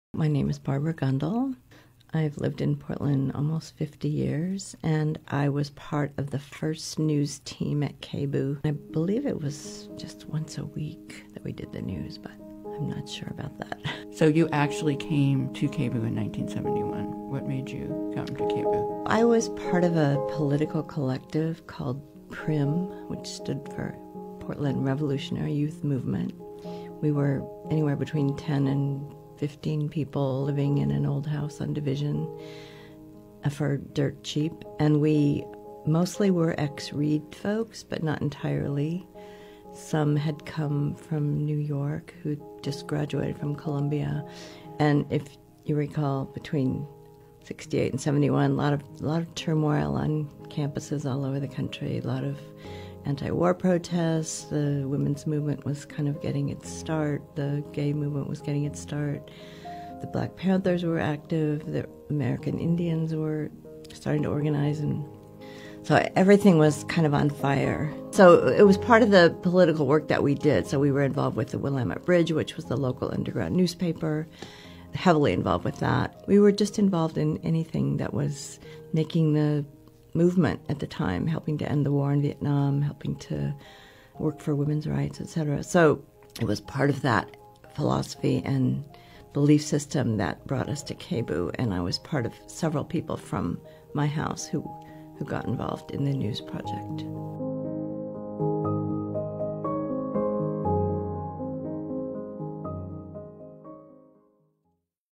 KBOO Stories Project: Barbara Gundle
Barbara Gundle was part of the first news team at KBOO in 1971. She and other members of the Portland Revolutionary Youth Movement were dedicated to political work and sharing information about social justice issues. For the full interview and many more like it go to the 50 years of KBOO website or for the full KBOO experience visit the 50 years of KBOO exhibit at the Oregon Historical Society.